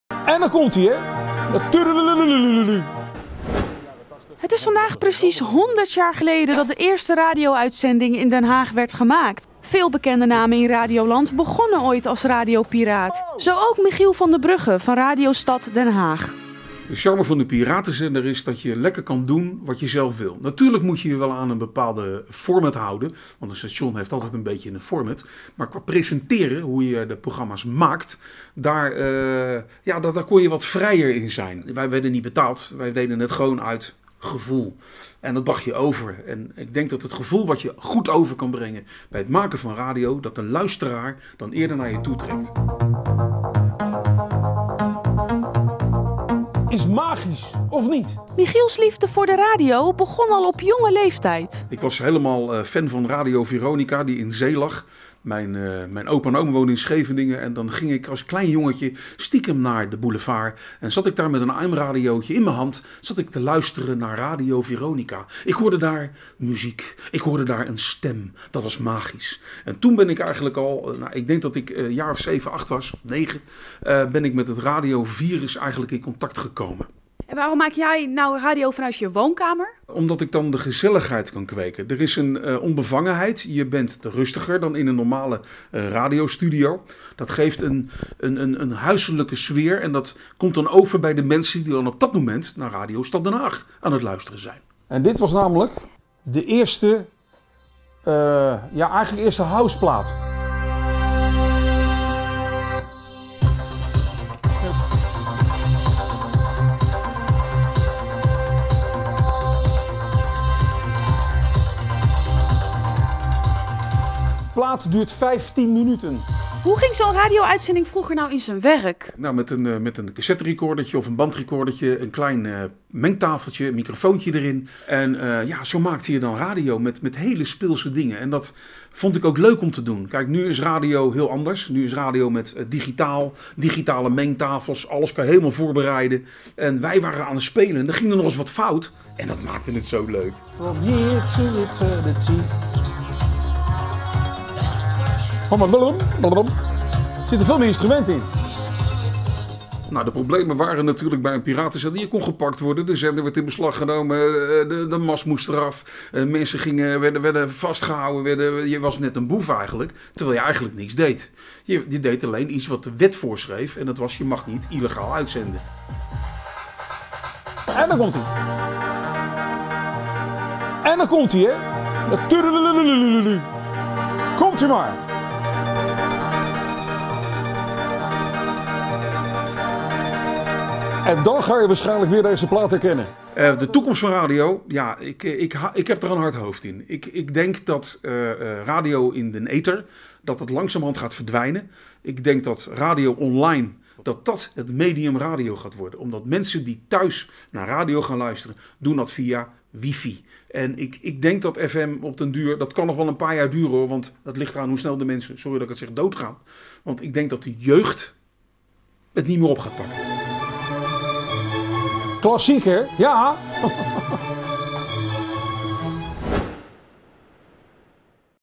100 jaar radio uitzendingen: portret Michiel van der Brugge
Het is woensdag precies 100 jaar geleden dat de eerste radio-uitzending in Den Haag gemaakt.
Veel bekende namen in radioland begonnen ooit als radiopiraat. Zo ook Michiel van der Brugge van Radio Stad Den Haag. Hij maakt al jaren vanuit zijn woonkamer radio, zonder regels en bemoeienis.